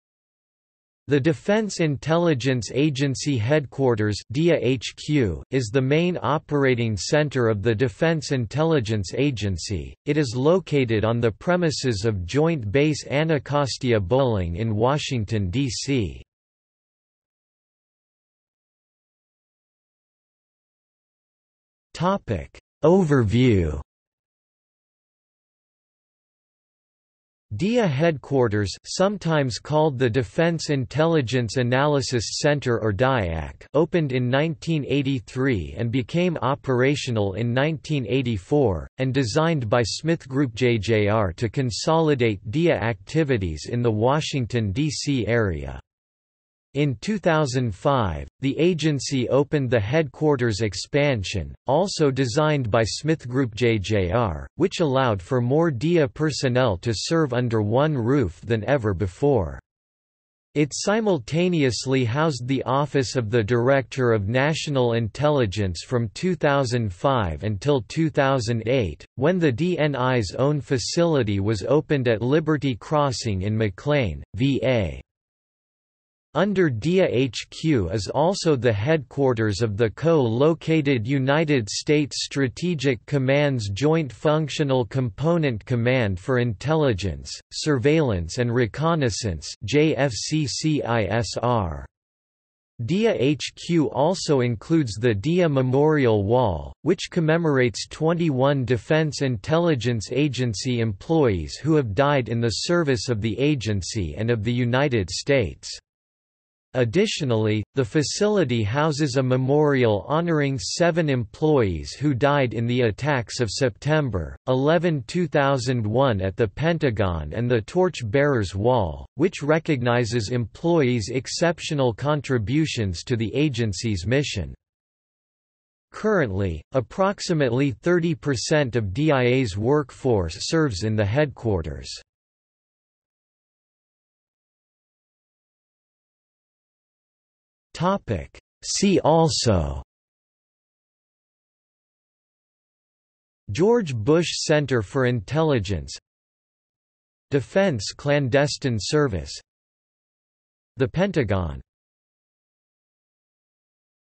Defense Intelligence Agency Headquarters | Wikipedia audio article
00:00:18 Overview
00:02:14 See also

- Socrates

SUMMARY
The Defense Intelligence Agency Headquarters (DIA HQ) is the main operating center of the Defense Intelligence Agency.  It is located on the premises of Joint Base Anacostia–Bolling in Washington, DC.